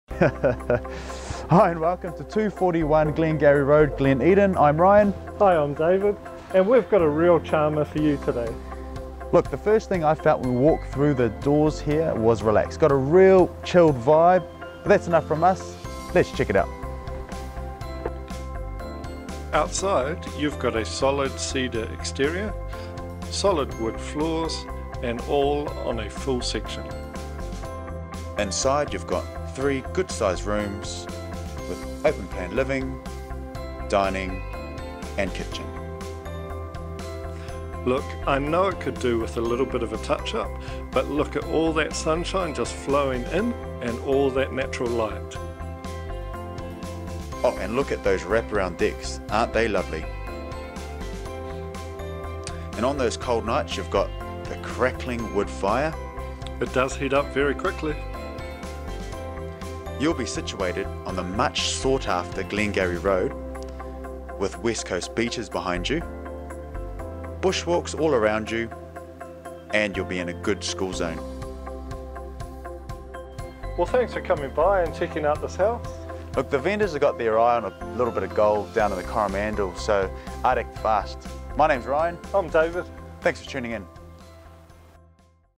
SOLD - 241 Glengarry Road, Glen Eden - Ryan Young
A REAL CHARMER - FAMILY HOME WITH POTENTIAL TO ADD VALUE!

Are you looking for a robust, Kiwi style home with a great 'family size' section which shouldn't break the bank!?

If you don't mind doing a little DIY, the benefits of this solid three bedroom ...

Property Marketed by: Ryan Young, Titirangi